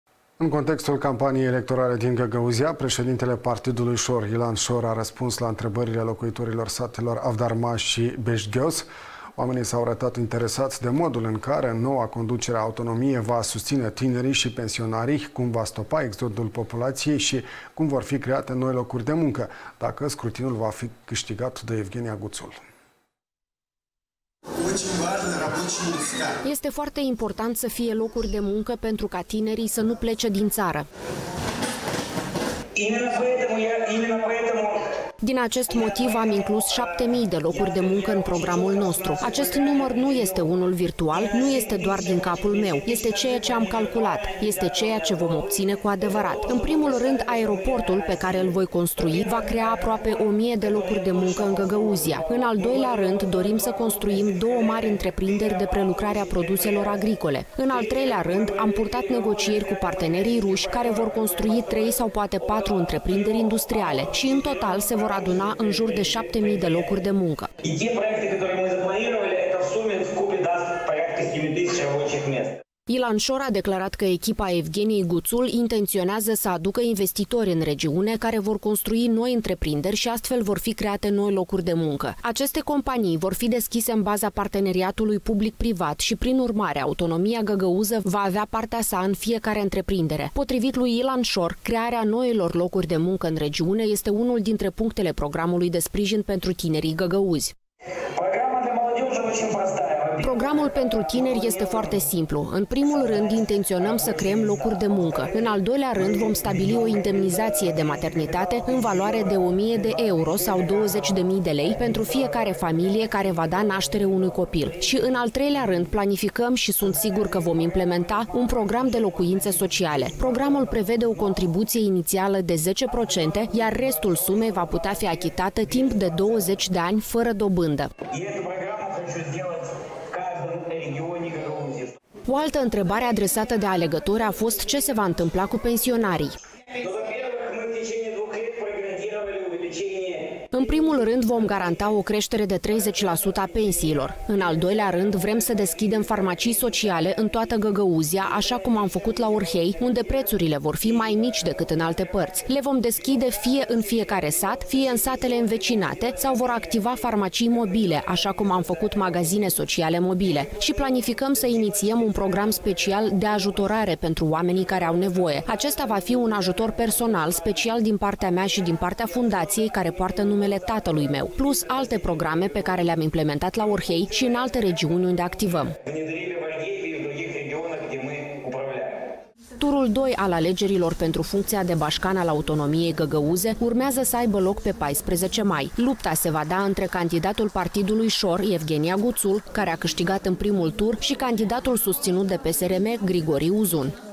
Campania din Găgăuzia
În contextul campaniei electorale din Găgăuzia, președintele Partidului "ȘOR", Ilan Șor, a răspuns la întrebările locuitorilor satelor Avdarma și Beșghioz. Oamenii s-au arătat interesați de modul în care noua conducere a autonomiei va susține tinerii și pensionarii, cum vor stopa exodul populației și cum vor fi create noi locuri de muncă.